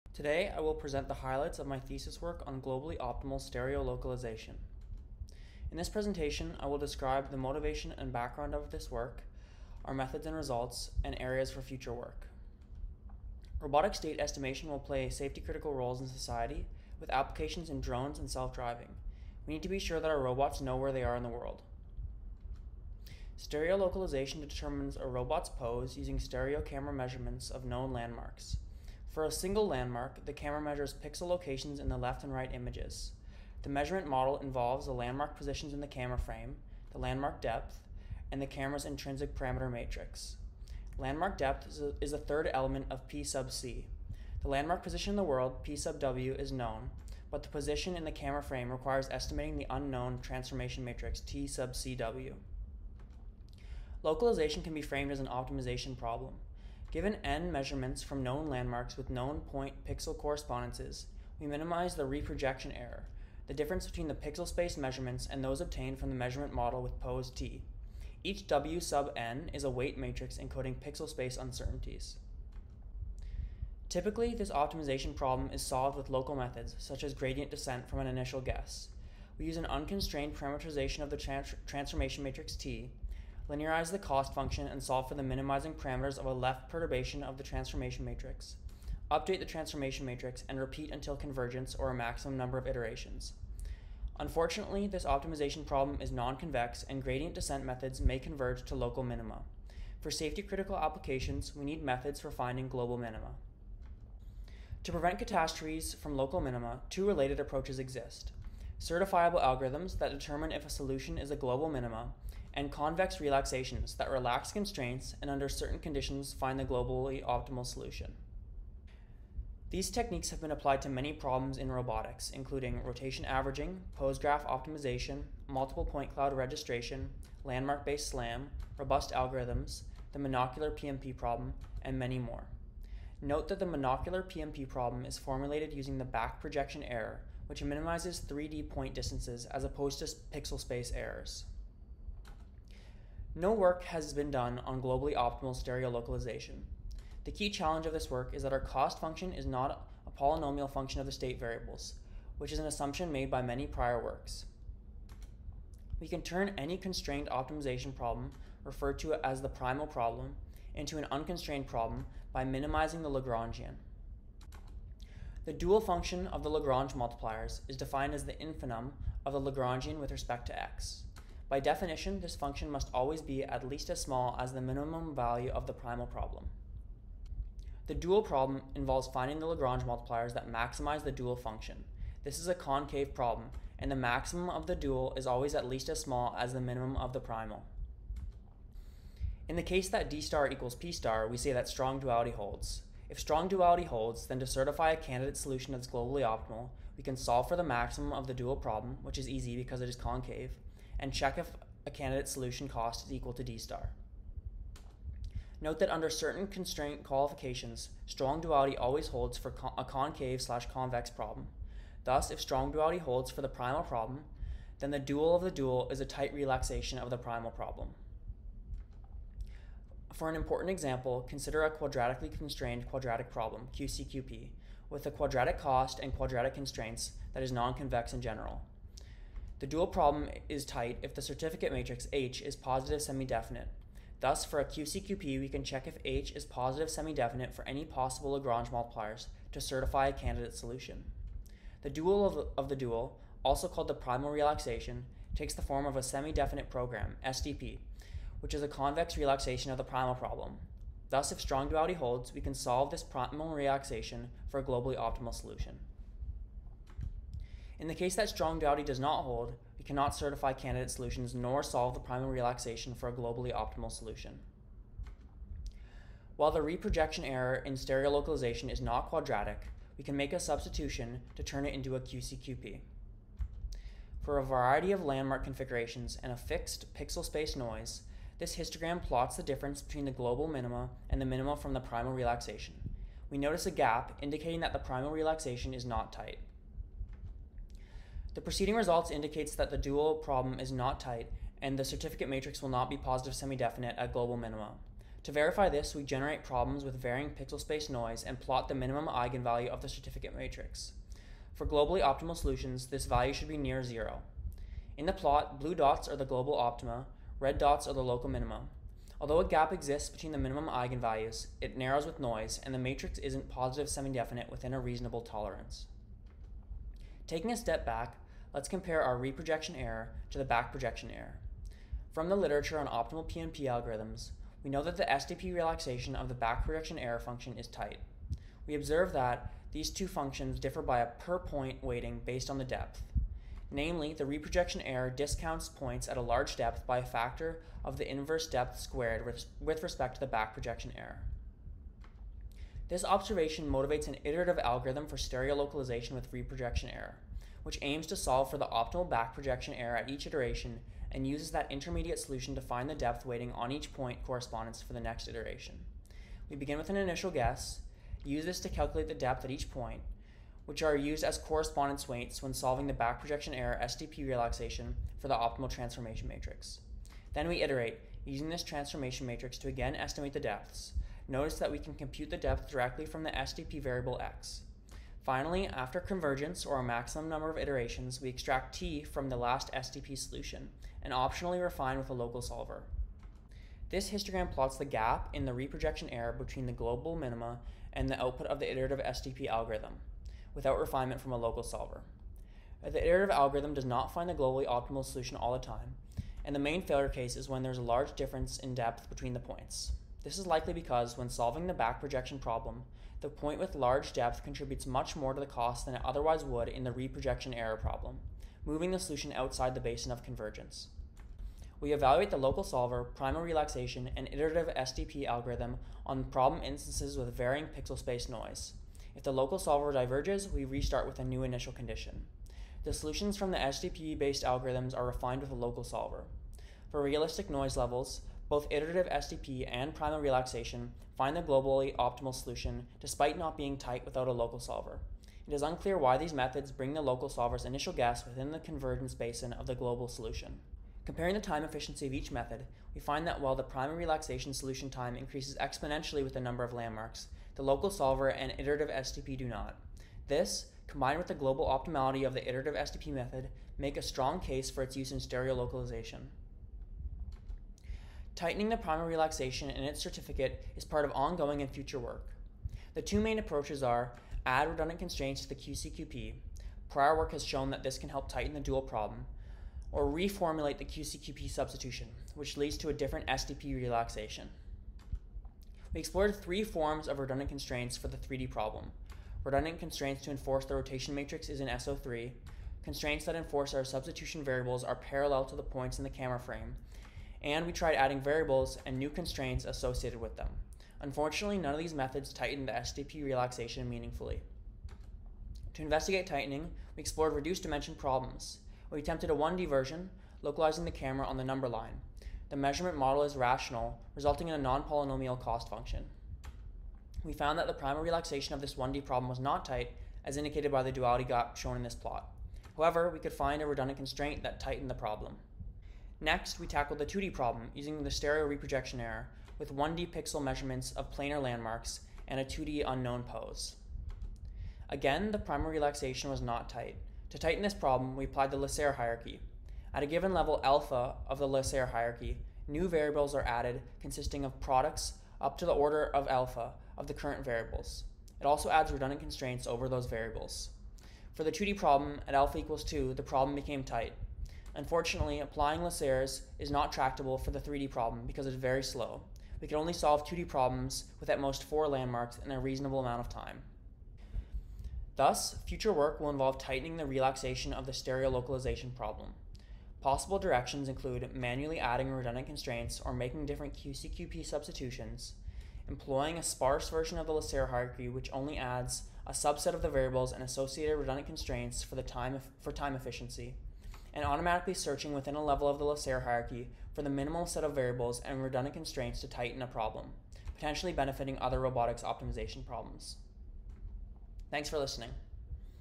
Towards Globally Optimal Stereo Localization
This is a presentation summarizing my undergraduate thesis work in Engineering Science at UofT.

Work done with Tim Barfoot, Connor Holmes, and Frederike Dümbgen.